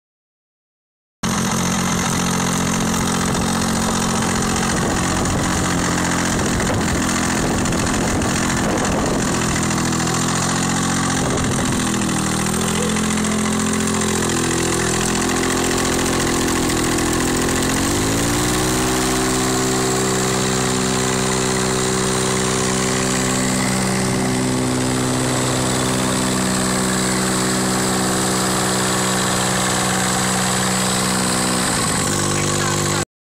A parajet paramotor powered by a rotary engine
Amazing machine, will just need lotto to afford the $12,000 price tag. Evan runs it up at the Tauramunui fly in